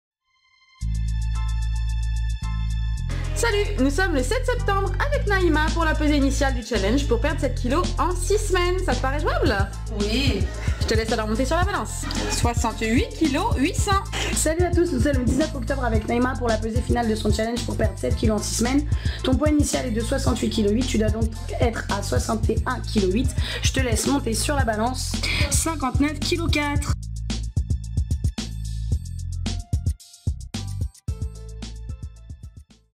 Transformation physique rapide Nantes
Naima a perdu 9,400 kgs en 6 semaines à la Training Academy